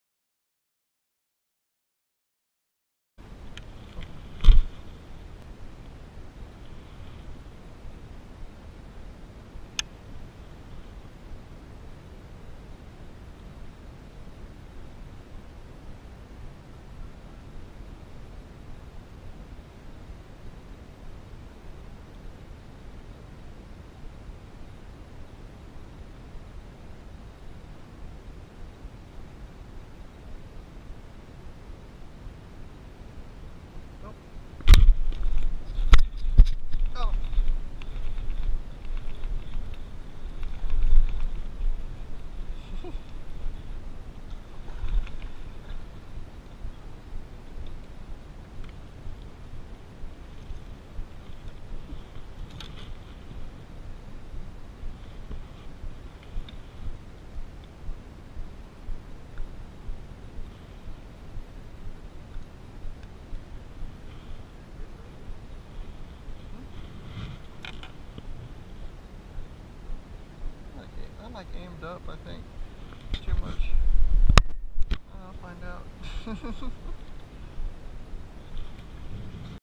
First GoPro Bass
Fishing deep for Large Mouth Bass in Lake Nacogdoches. It was my first time using my new GoPro+.